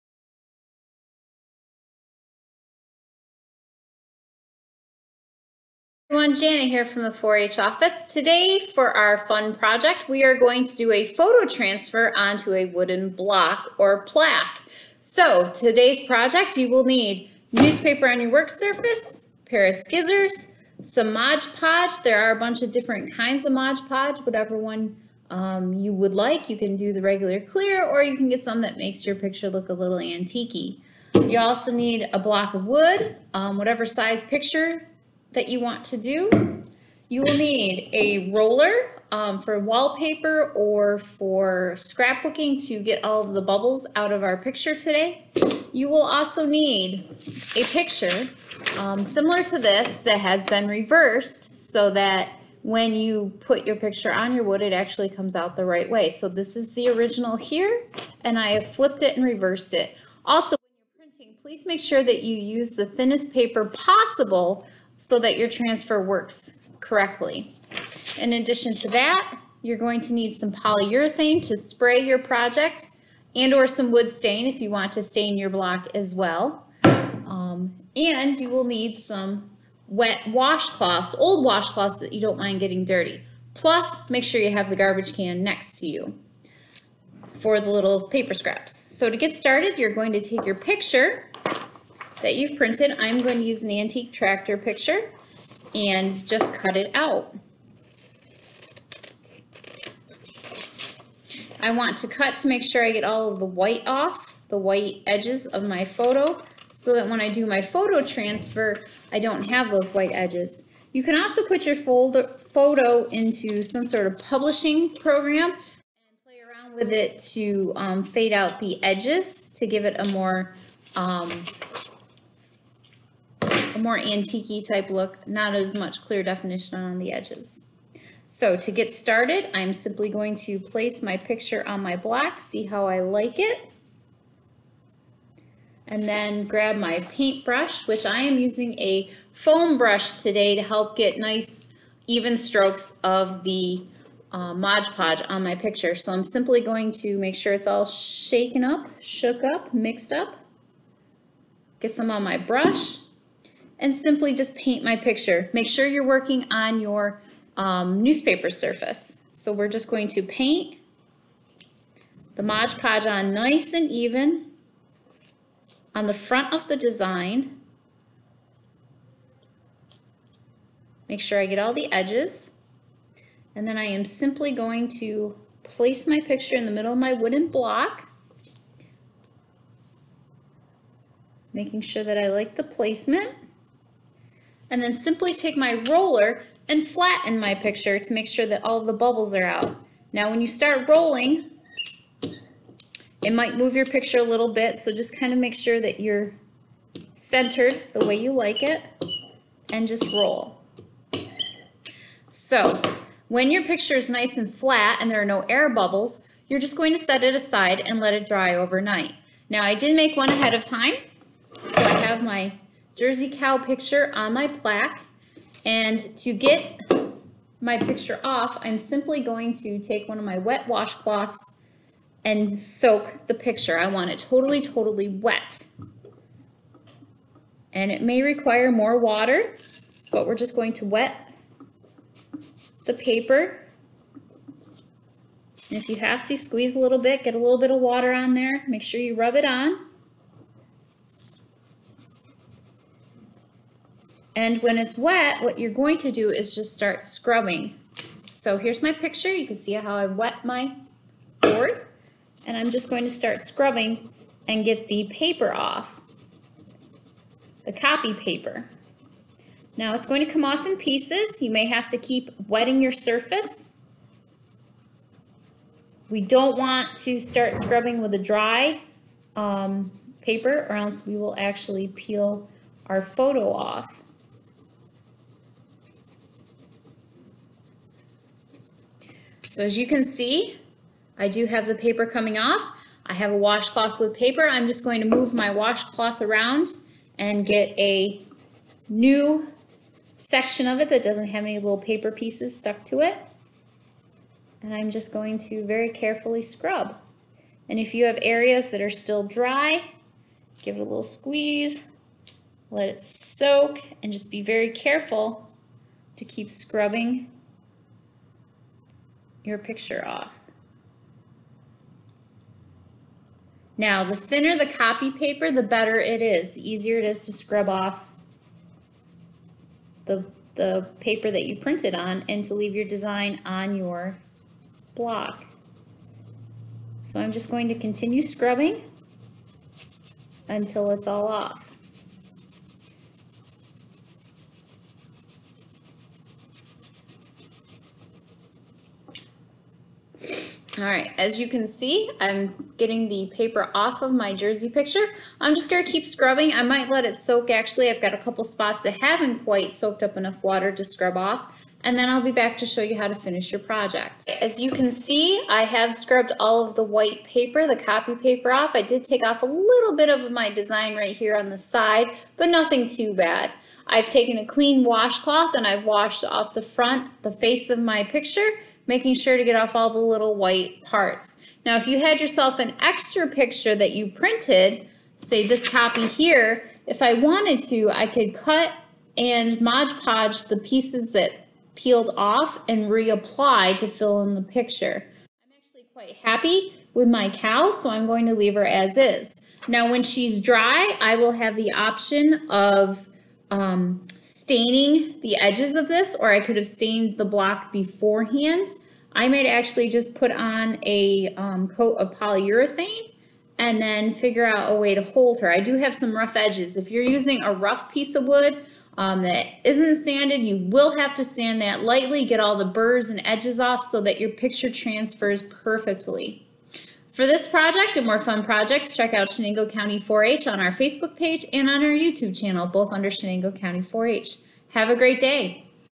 Photo Block
Learn how to take one of your cherished family photos, print it out from your home printer and transfer it a wooden plaque/block in 24 hours!